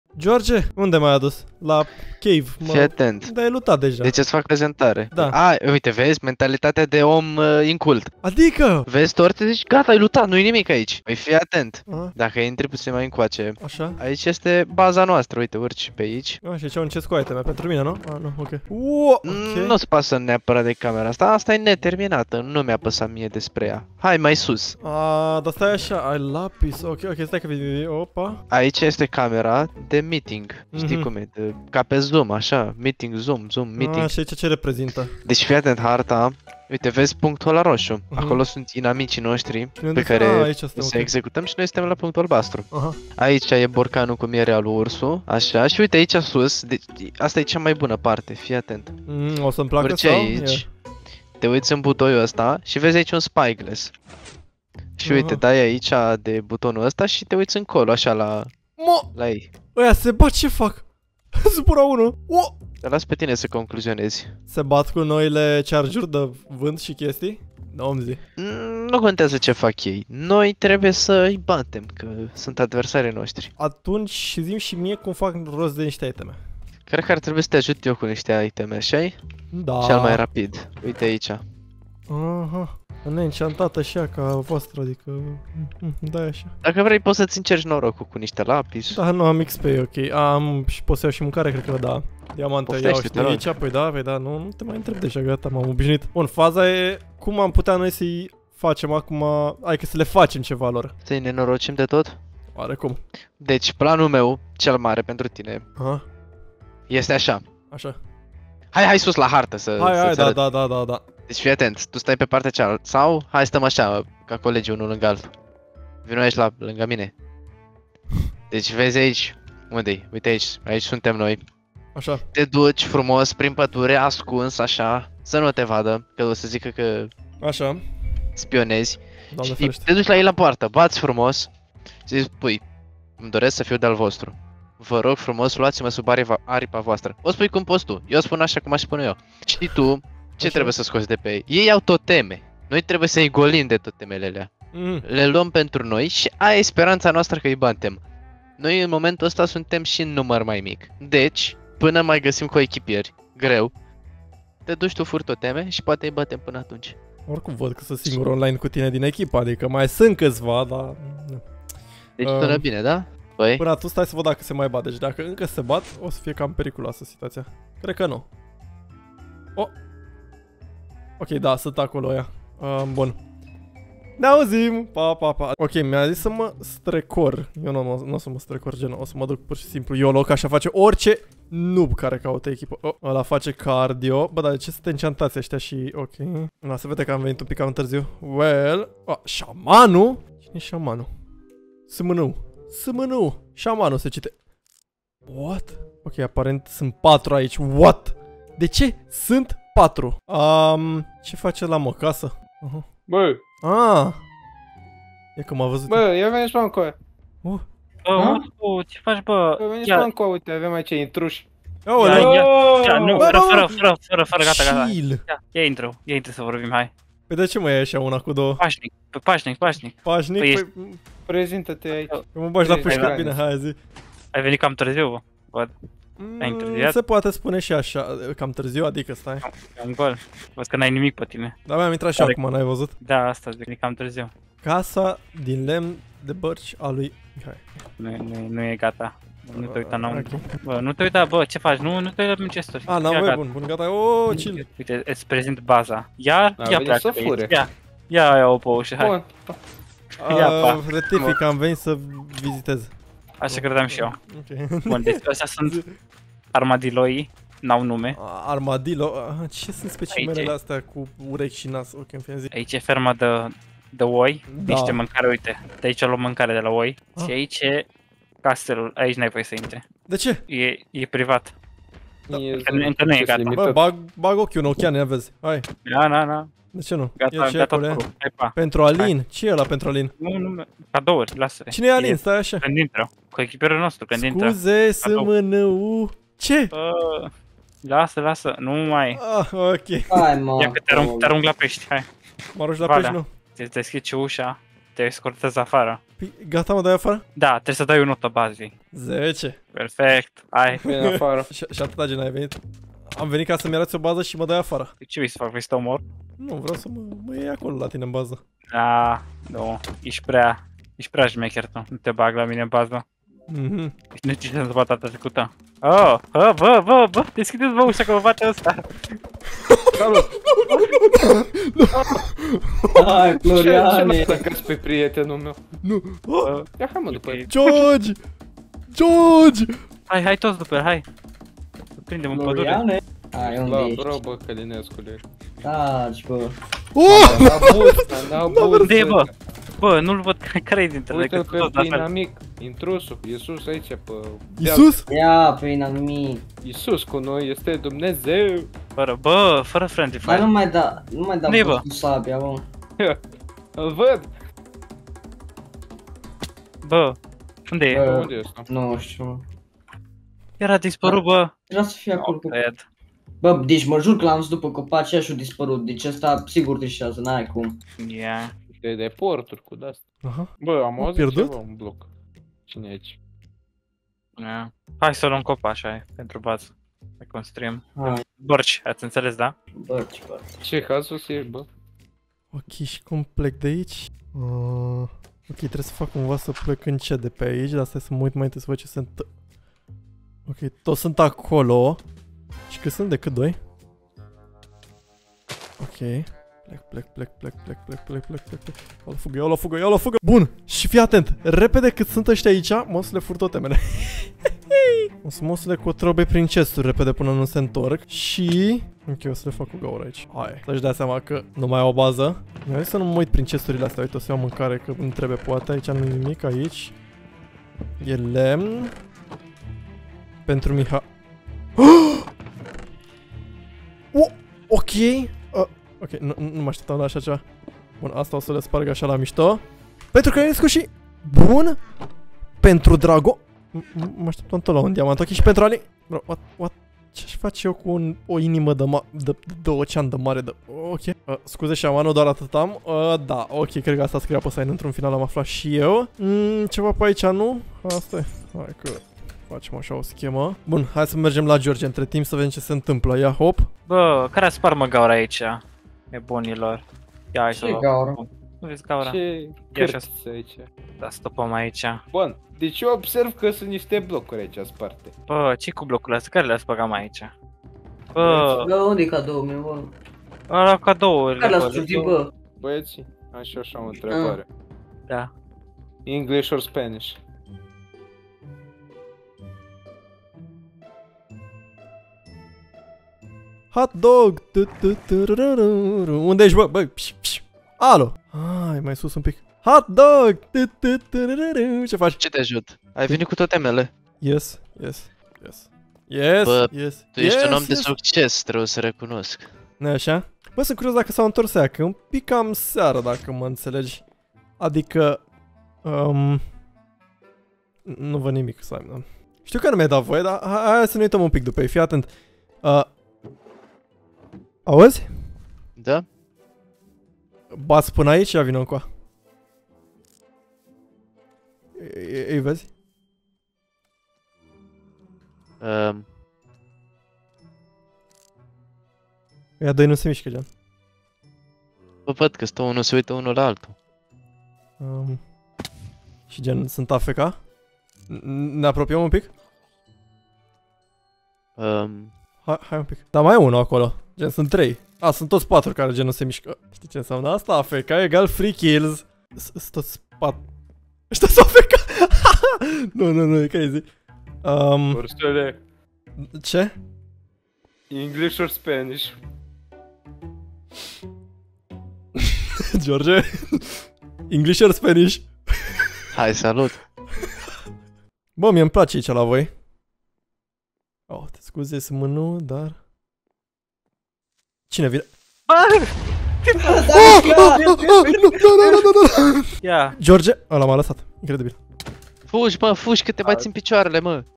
Am Gasit *CASTELUL INAMICILOR* de pe NOUL HARDCORE 1.21
Am *GASIT CASTELUL* de pe NOUL HARDCORE 1.21